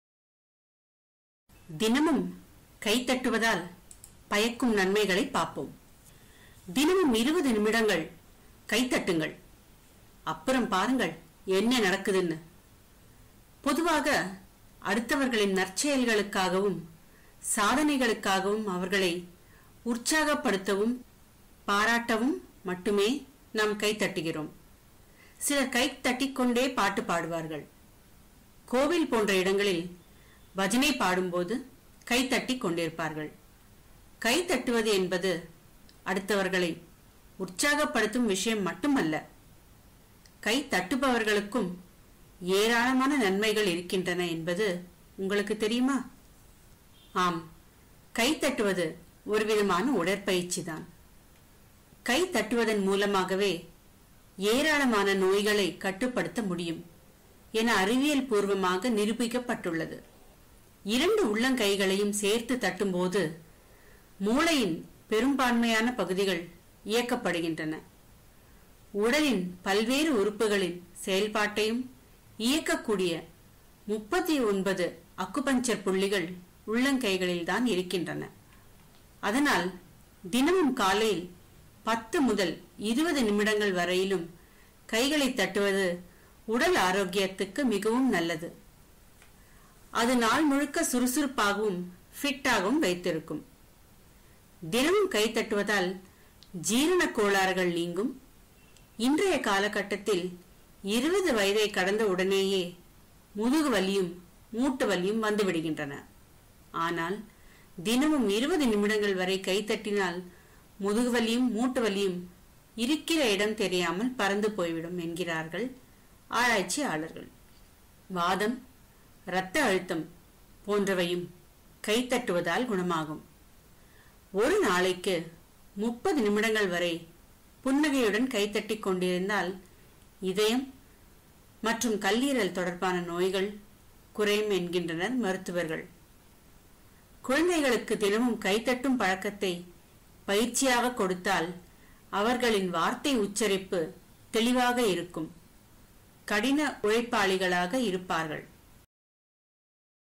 கை தட்டுதலின் மகிமை-Clapping of Hands
Clapping of hands usually signifies to encourage people.

It also has health benefits such as stremling of Digestive Disorder, bowel issues, acidity, etc.

Best Cure for back pain, neck pain, joint pain.

It helps in treating Gout and Arthritis, low blood pressure.

If clapping of hands is practiced among children like exercise everyday, they tend to become very active and their speaking skills improves.